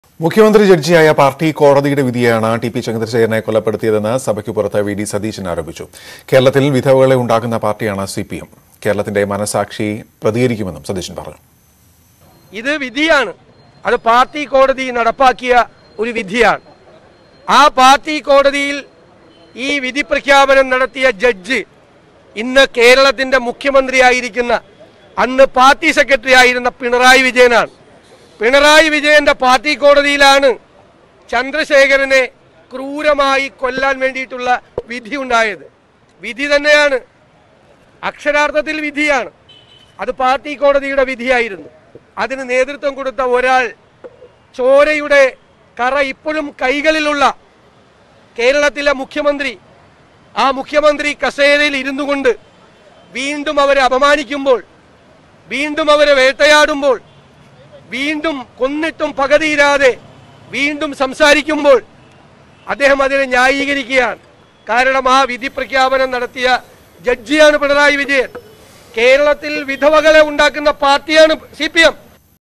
"മുഖ്യമന്ത്രി ജഡ്ജിയായ പാർട്ടി കോടതിയുടെ വിധിയാണ് TPയുടെ ജീവനെടുത്തത്": V D Satheesan
Kerala Assembly Session : KK Remaക്കെതിരായ MM Maniയുടെ പരാമർശത്തിനെതിരെ ഇന്നും സഭയിൽ പ്രതിപക്ഷ പ്രതിഷേധം. MM Mani മാപ്പ് പറയണമെന്നാണ് പ്രതിപക്ഷത്തിന്റെ ആവശ്യം. പ്രതിഷേധത്തെ തുടർന്ന് ചോദ്യോത്തര വേള റദ്ദാക്കി.

About the Channel:
| n18oc_kerala